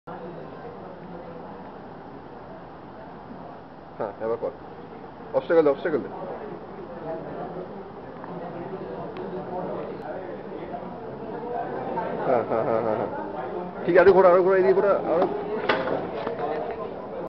obstacle avoider robot @ iiserk workshop
disaster during the demonstration!